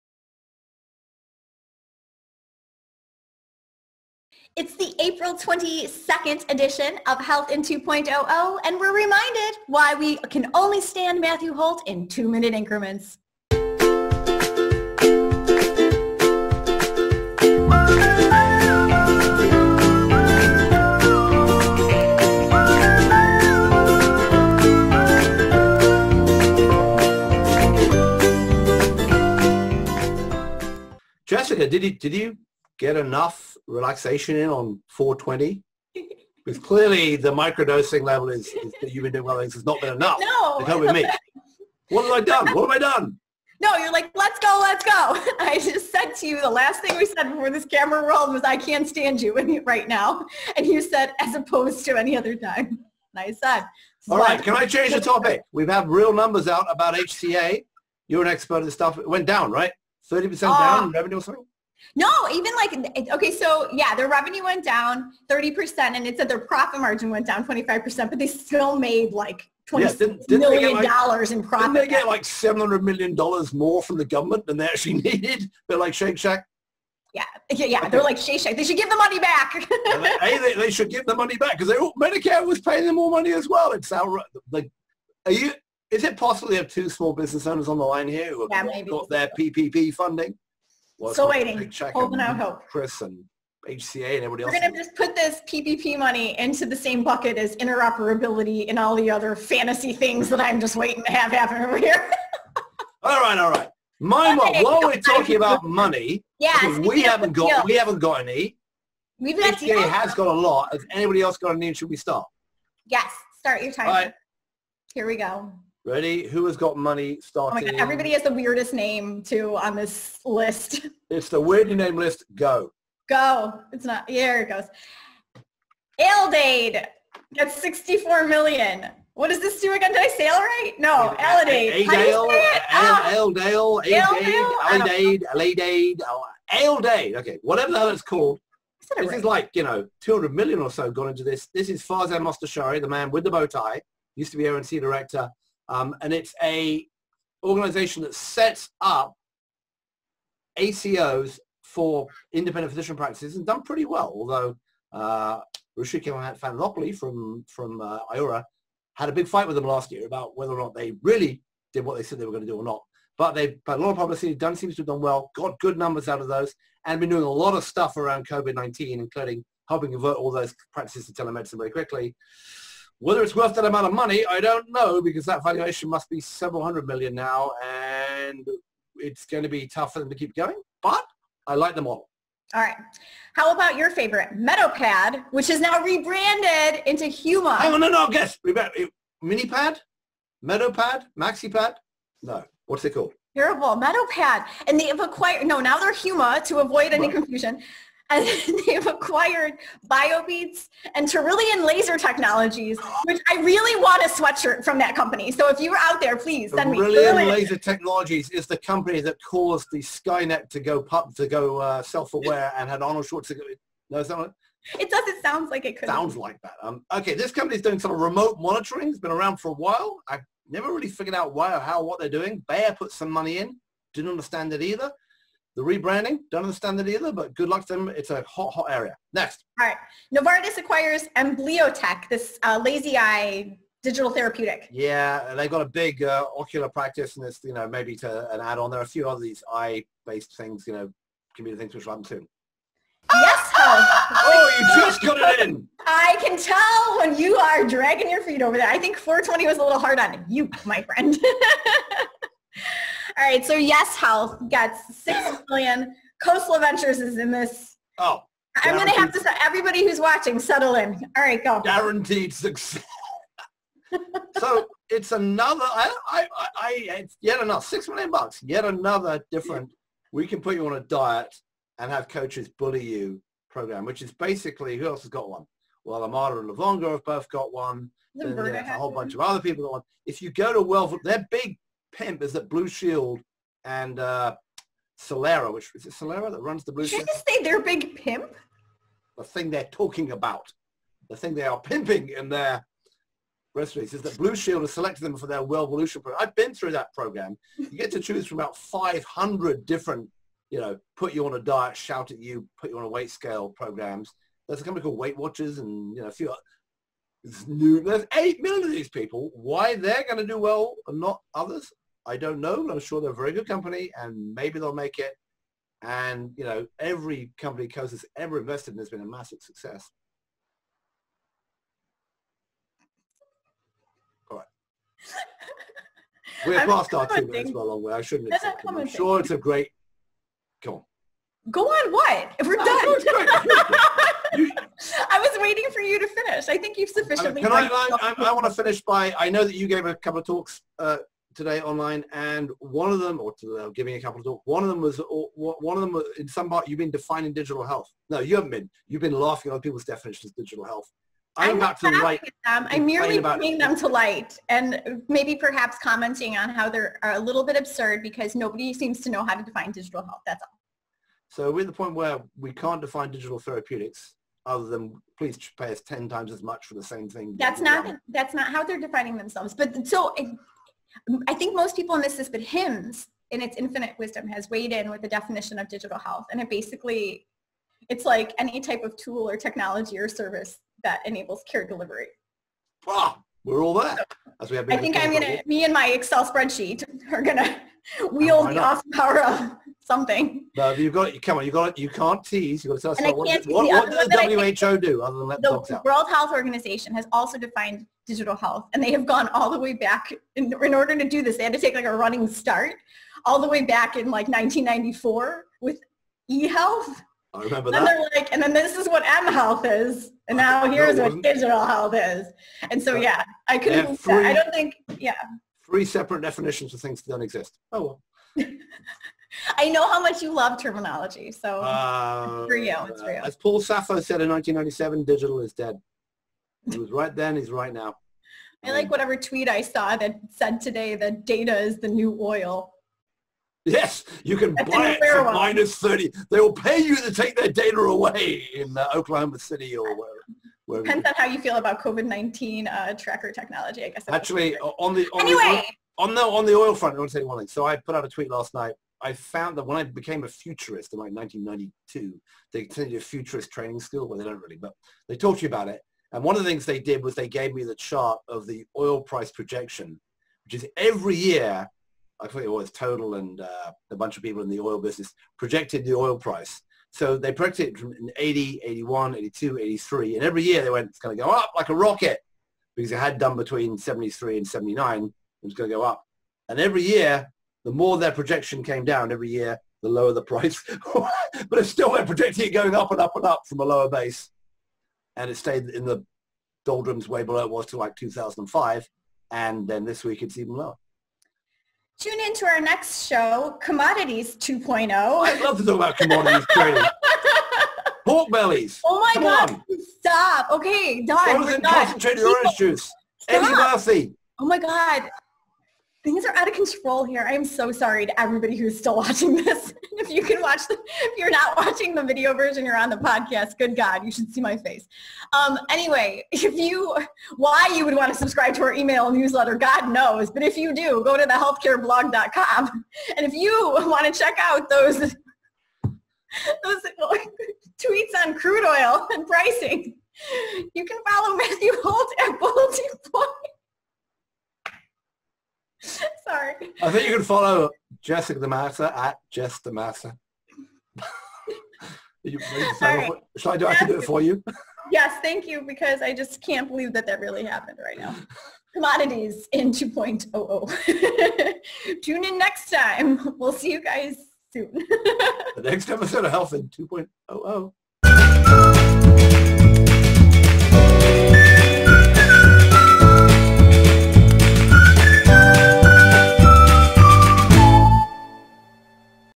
Healthin2Point00, Episode 118 | Aledade, Medopad, Amblyotech and Yes Health
Today on Health in 2 Point 00, Jess and Matthew talk about HCA now that the real numbers have come out. Jess asks Matthew about Aledade raising $64 million. Founded by former ONC director Farzad Mostashari, they set up ACOs for independent physician practices and have been doing a lot around COVID-19. Medopad has rebranded as Huma and acquired Biobeats and Tarilian Laser Technologies (TLT); they've been doing remote monitoring and have been around for a while. Novartis acquires Amblyotech, a lazy eye digital therapeutic. Finally Yes Health gets $6 million - yet another "we'll put you on a diet and have coaches bully you" platform.